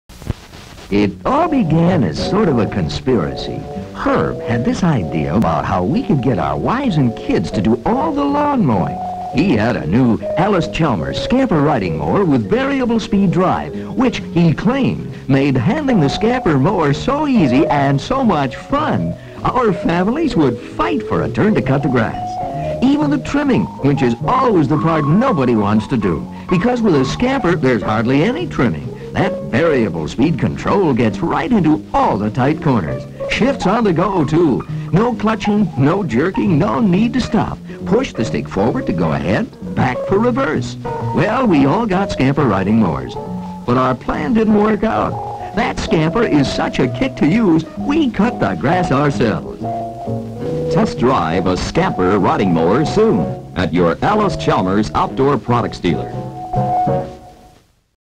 1960's Allis Chalmers Scamper Lawn Mower TV Commercial
1960's TV Commercial for the Allis Chalmers Scamper Lawn Tractor.